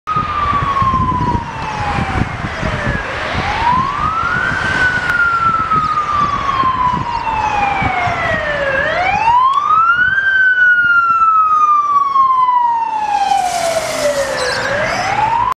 Tasmania Fire Service - Hobart 1.2 Responding
Hobart 1.2 (Heavy Pumper) responding to a residential fire incident in Tranmere. Hobart 1.2 was filling in station 3 (Clarence) for Clarence 1.1 that was on a brush fire call.